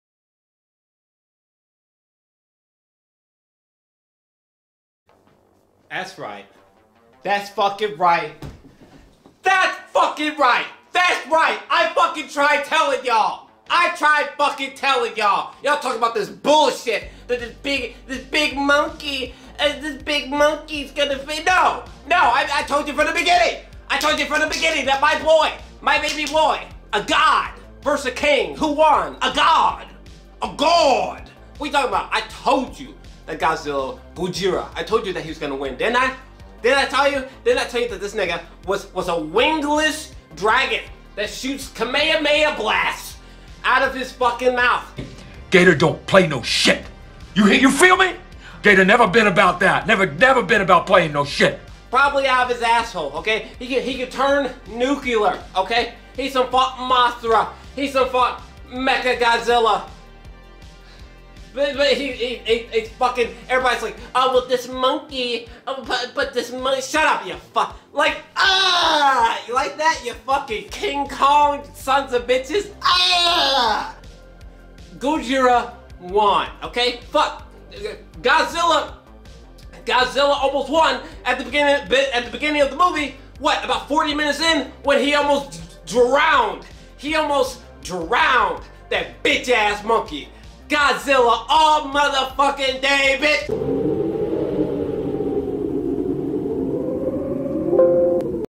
******** Won.
Godzilla vs. Kong Movie: My updated thoughts on who WON.

A 2021 American monster film directed by Adam Wingard.

 A sequel to Godzilla: King of the Monsters (2019) and Kong: Skull Island (2017), it is the fourth film in Legendary's MonsterVerse.

The film is also the 36th film in the Godzilla franchise, the 12th film in the King Kong franchise, and the fourth Godzilla film to be completely produced by a Hollywood studio.

The film stars Alexander Skarsgård, Millie Bobby Brown, Rebecca Hall, Brian Tyree Henry, Shun Oguri, Eiza González, Julian Dennison, Kyle Chandler, and Demián Bichir.

Scheduled to be released in the United States on March 31, where it will be released simultaneously in theaters and on HBO Max.